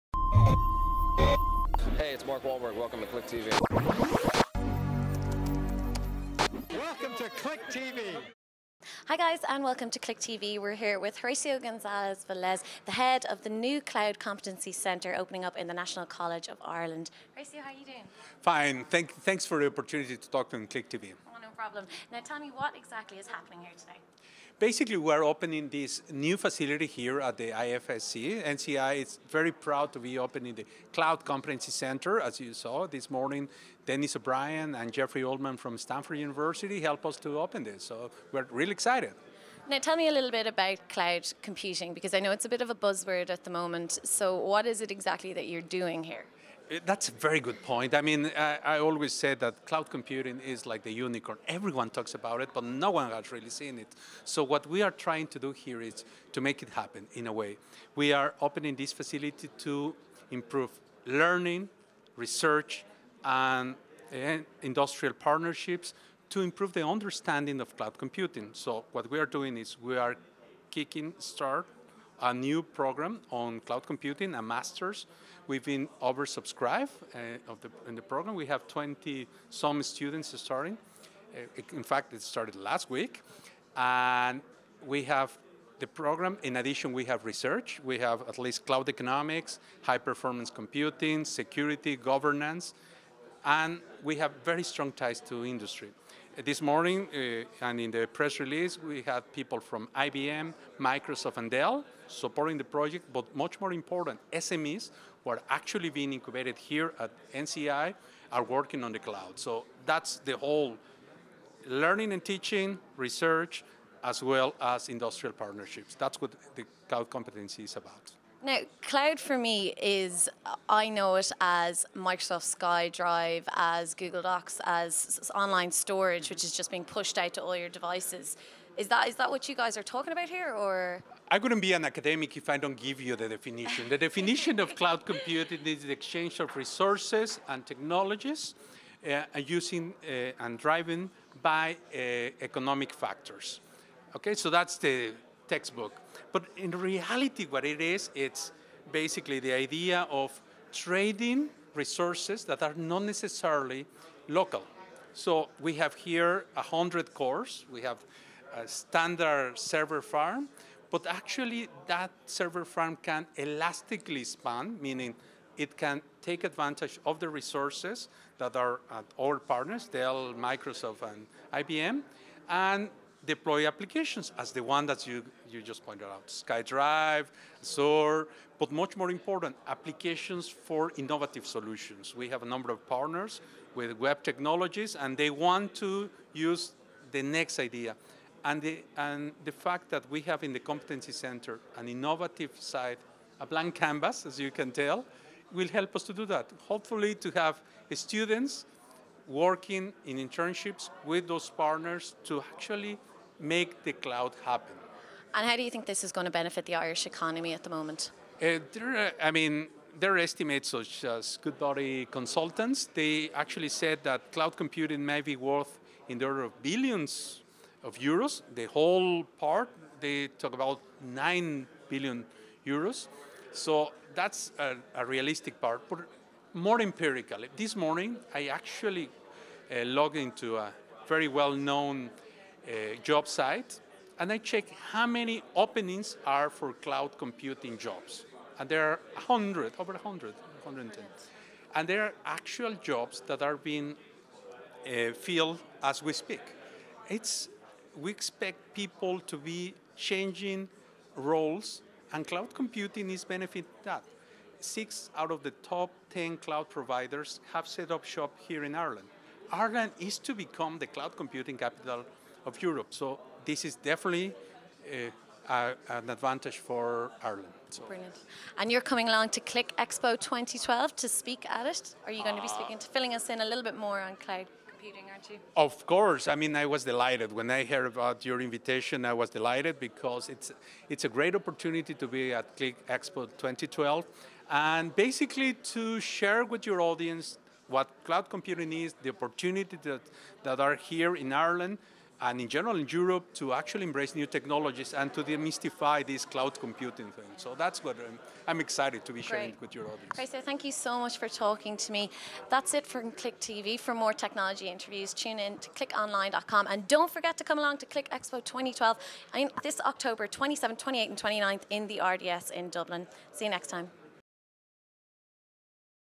What is Cloud Computing?
All the latest Movie news, reviews, interviews and previews!

Cloud Computing is one of those buzz words floating about the world today and if you're anything like us then you're not too convinced by what exactly all the fuss is about.   We went along to the recent opening of the new Cloud Competency Centre in the National College of Ireland to talk with Horacio Gonzalez Valez about what Cloud Computing means for our future and economy.